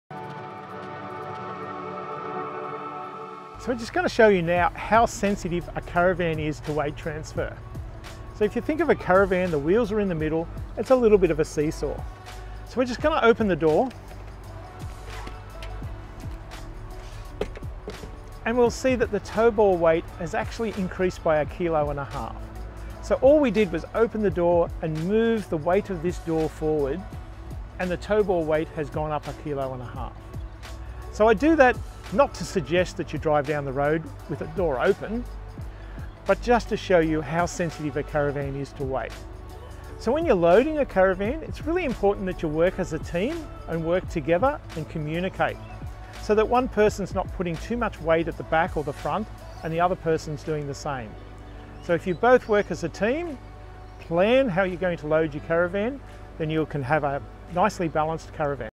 Tow ball weight. See how the tow ball weight of a caravan changes when we open the caravan door !
Jeff from CHECK WEIGHT Mobile Caravan Weighing explains how sensitive the tow ball weight is to weight transfer.
Just by opening the caravan door the tow ball weight changes.
Caravan Weights can be confusing however CHECK WEIGHT - the experts in caravan weights help make t easy.

CHECK WEIGHT weighs caravans - they are a mobile weighbridge.